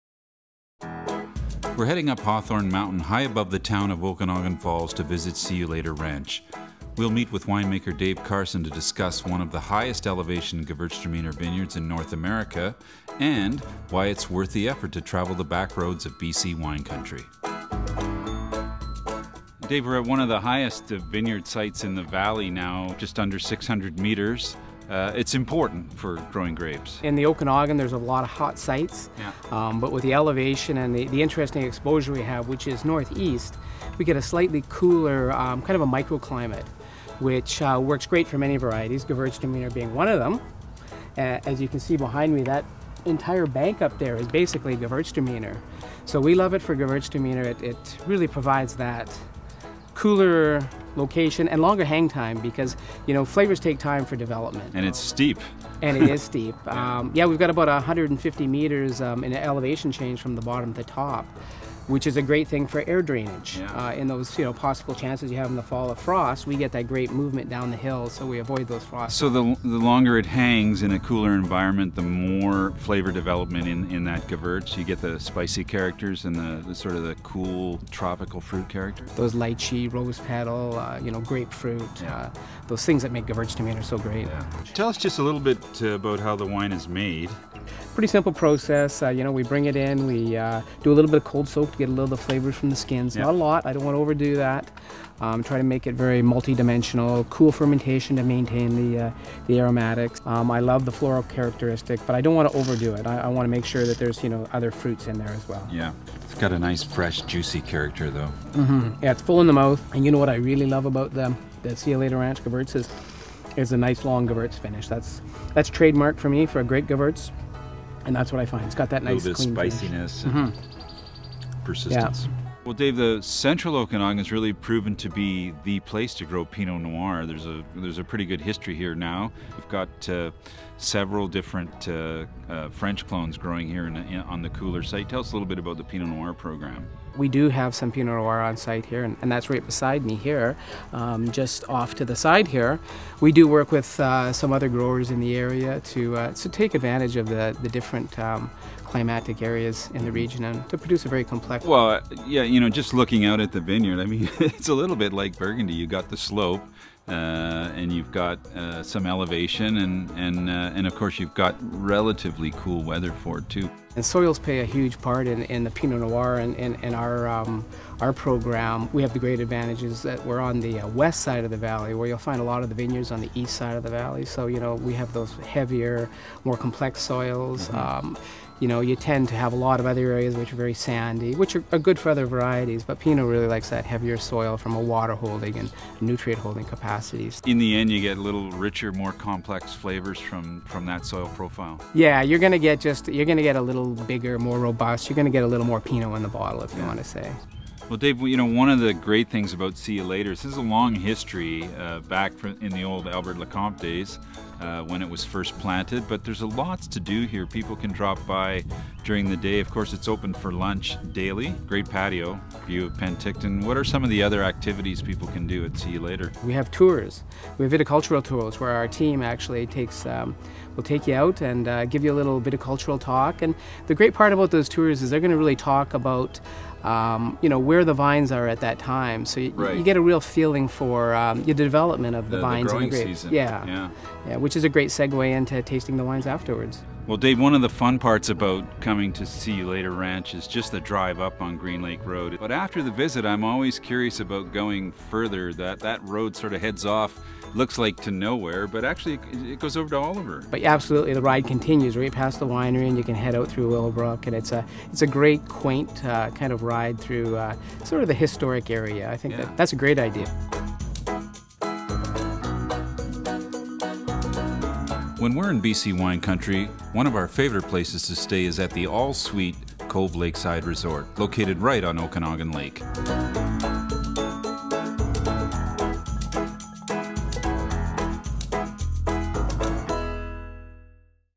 See Ya Later Ranch
See Ya Later Ranch was founded by Major Hugh Fraser in 1919. It was named after his London bride who upon seeing the rustic surroundings left both the Valley and a three-word note in a frying pan - “See ya later.” Today, winemaker Dave Carson, oversees one of the oldest and highest vineyard sites in all of the Okanagan Valley planted to pinot noir, riesling and the largest single plot of gewürztraminer (65 acres) in North America. Ride with us as we visit See Ya Later Ranch along one of the great backroads of British Columbia wine country.